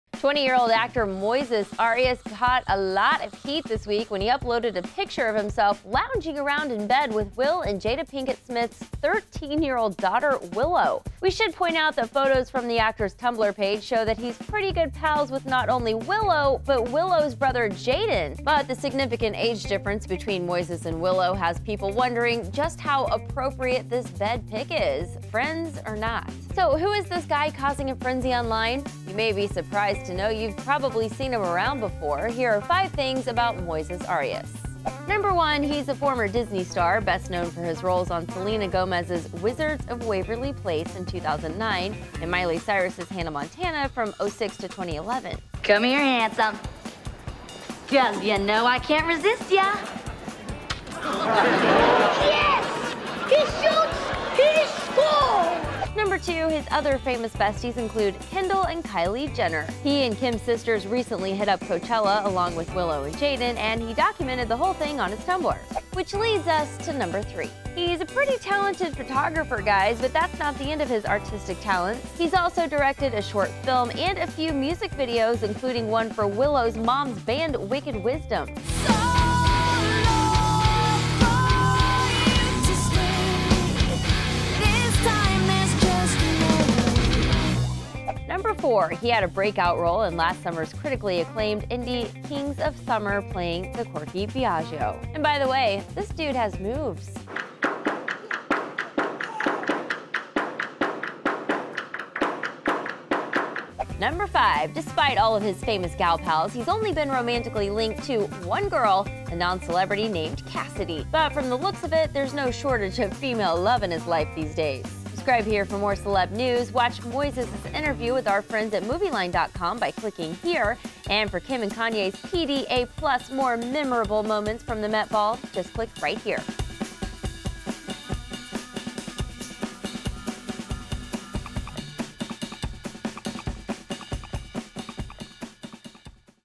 5 Important Things To Know About Moises Arias
Moises Arias posted a picture of himself with Willow Smith in bed recently. Here's five important facts about him.